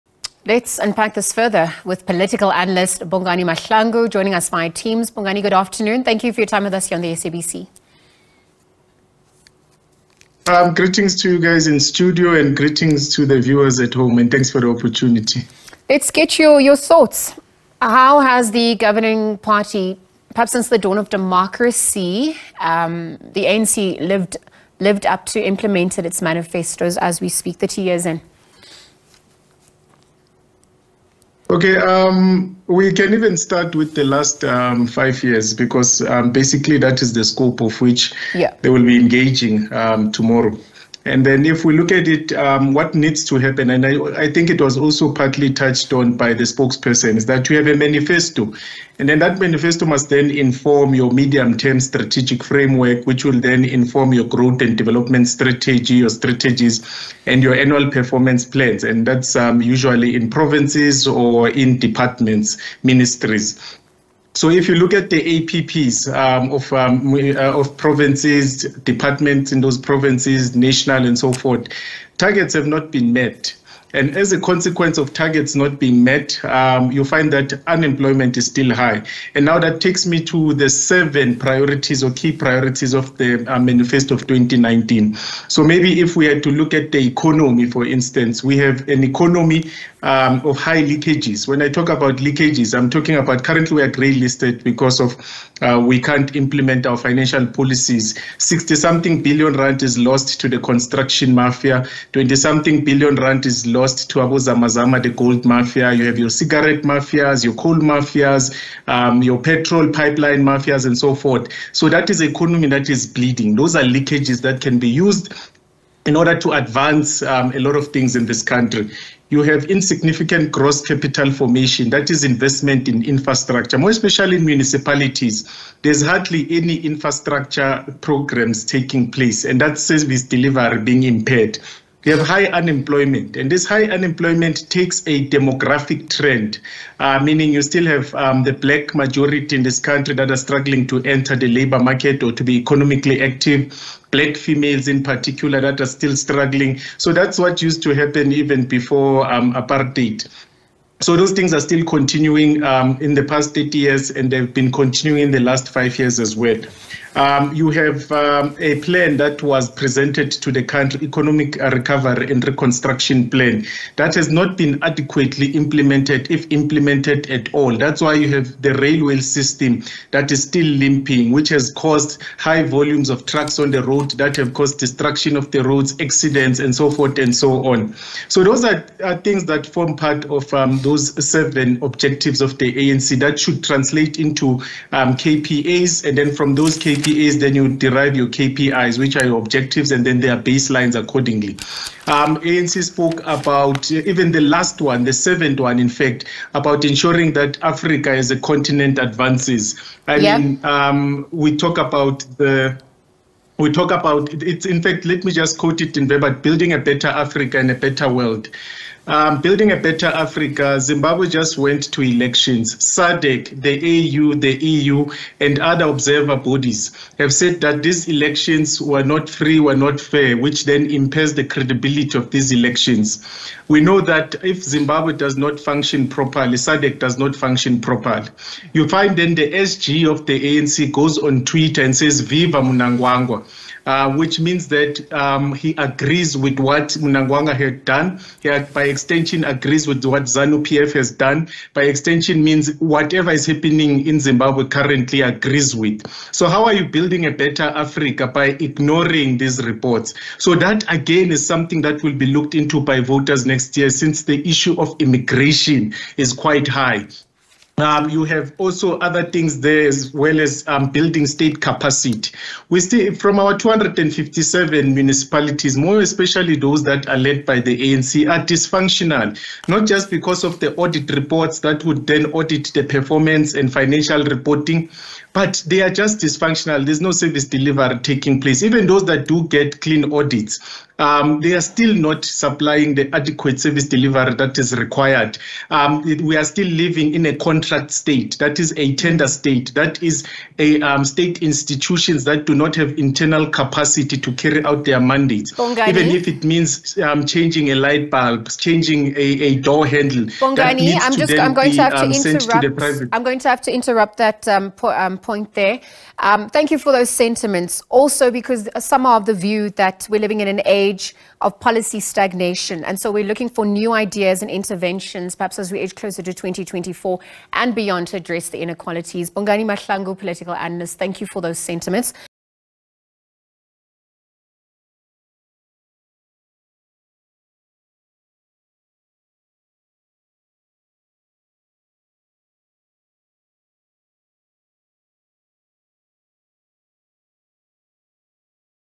ANC Manifesto Review discussion with Bongani Mahlangu
Let us unpack this further with political analyst Bongani Mahlangu.